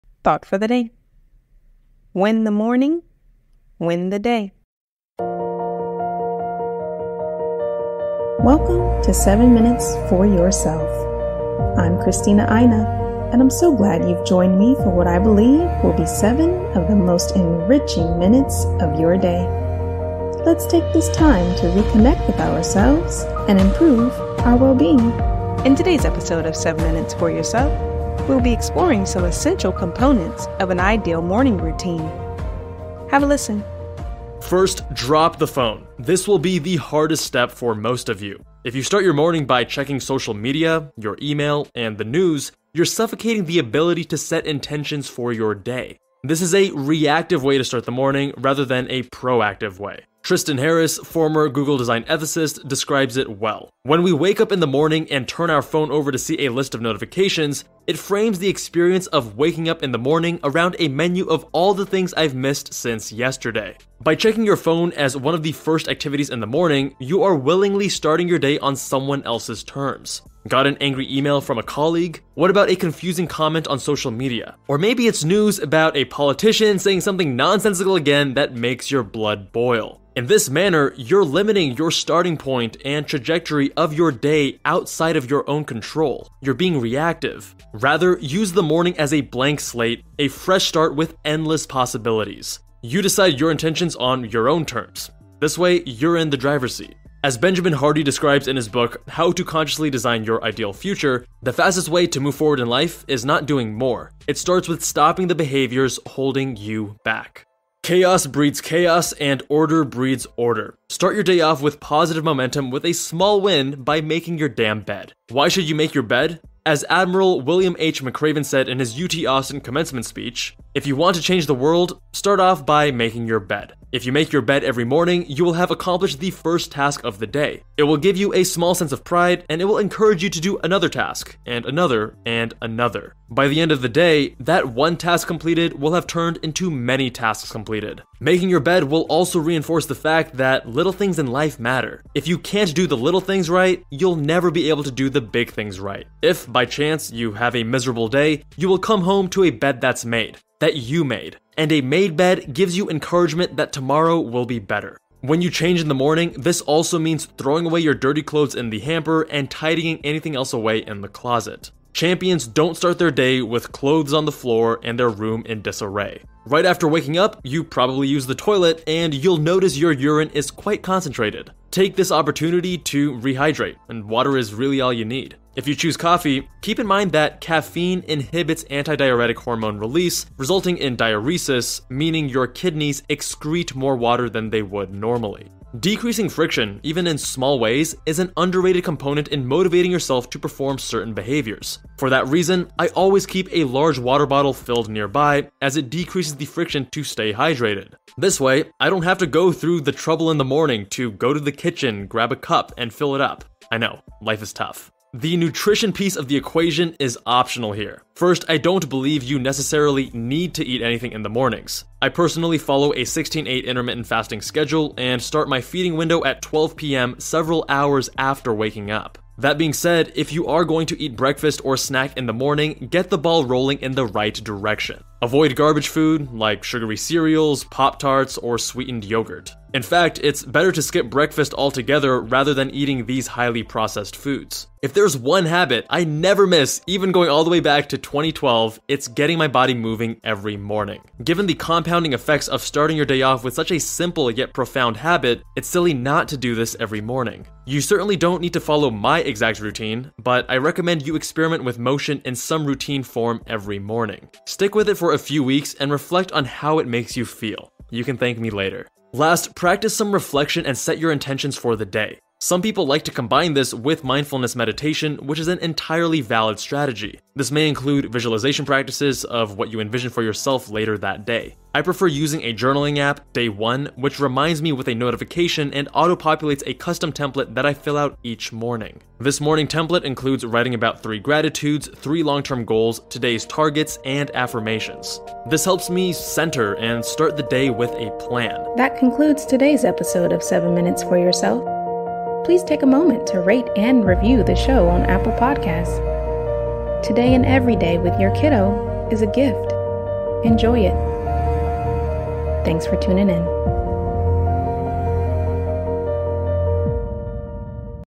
Morning Rituals for a Productive Day
Welcome to 7 Minutes for Yourself! Today, we're diving into the key components of a perfect morning routine.

Join us as we share tips and tricks to start your day on a positive and purposeful note.

Don't miss out on this valuable insight - tune in now!

The audio clip featured in today's episode is courtesy of our friends at Med School Insiders